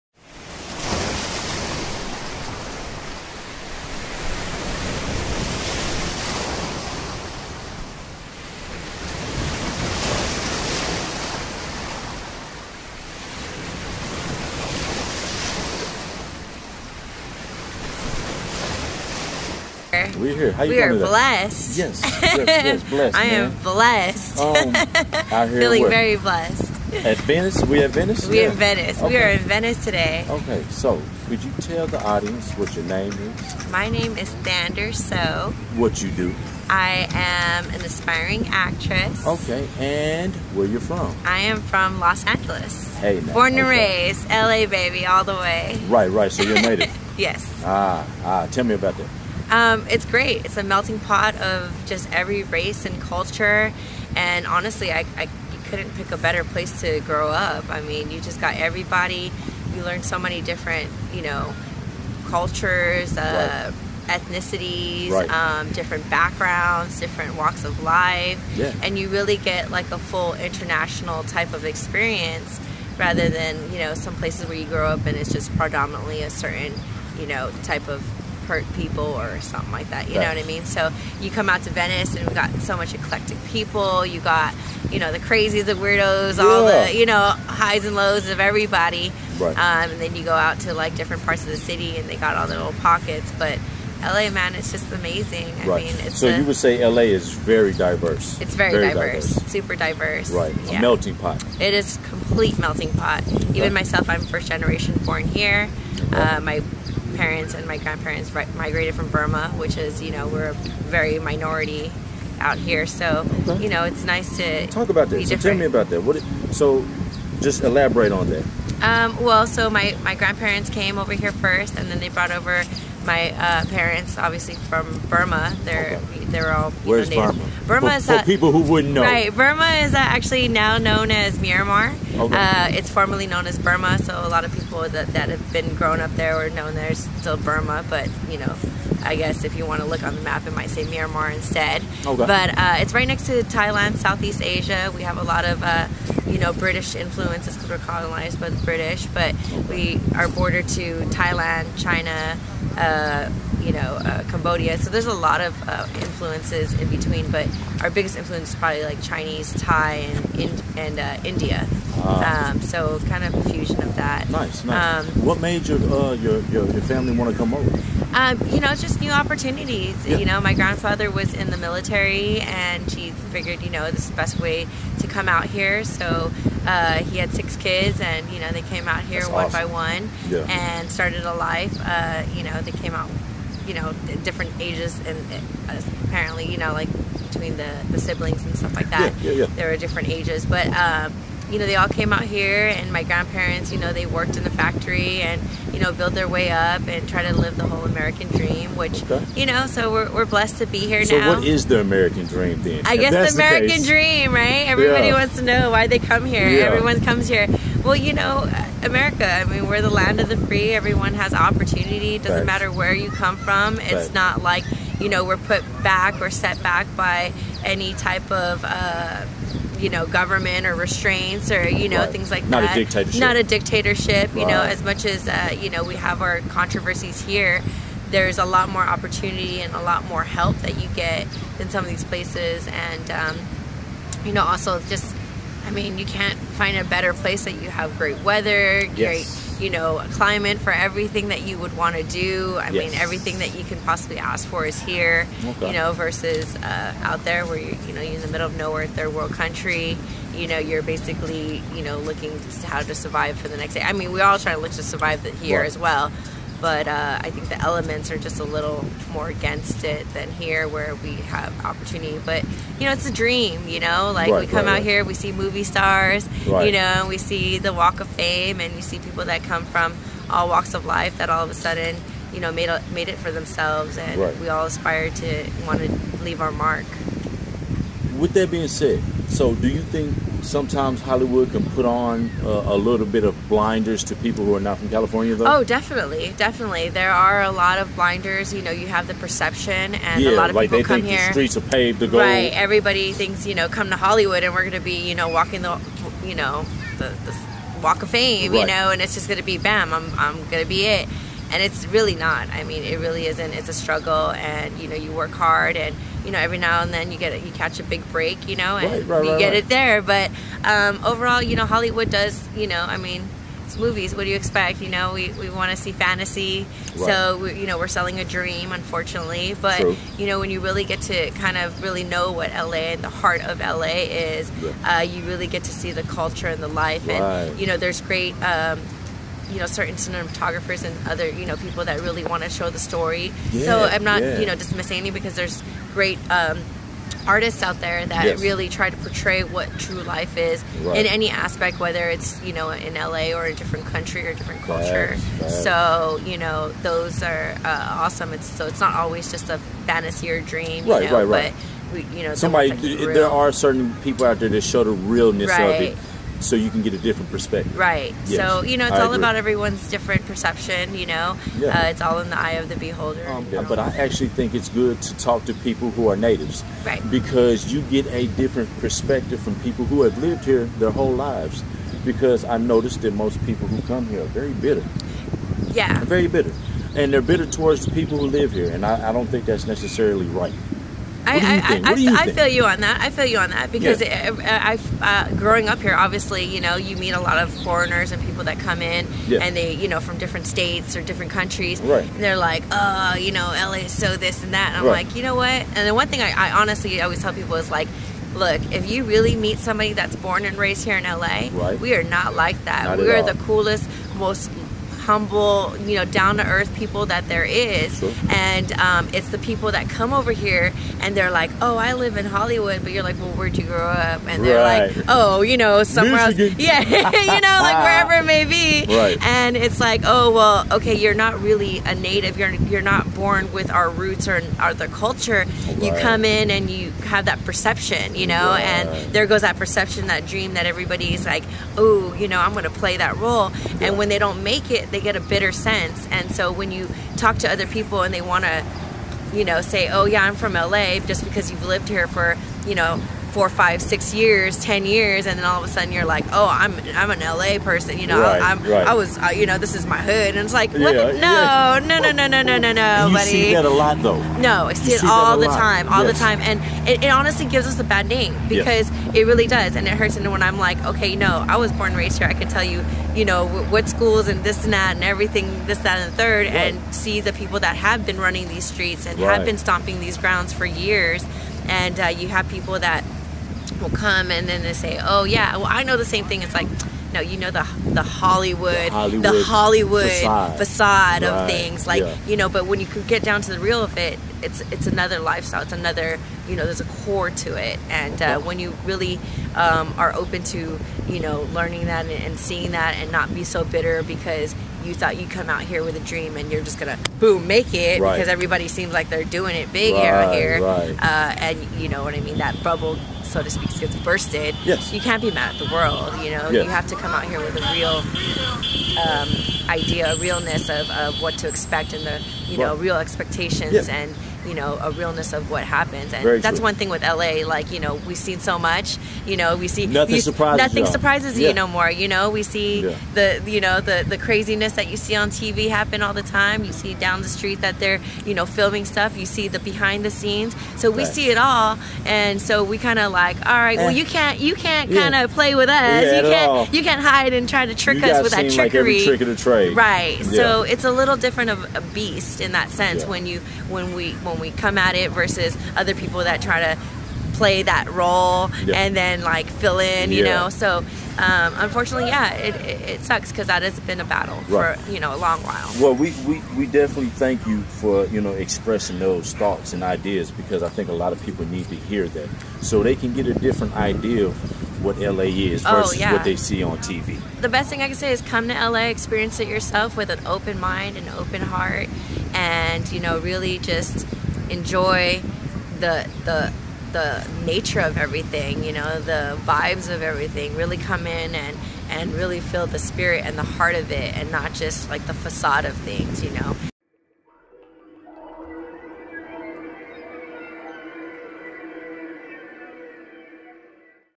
Interview with Actress Thander Soe | Hollywood lies!!!
Back again with another interview talking to the wonderful Actor Thander Soe. Her story about whats it's like to be a LA native, and if Los Angeles gets  a bad rep because of outsiders.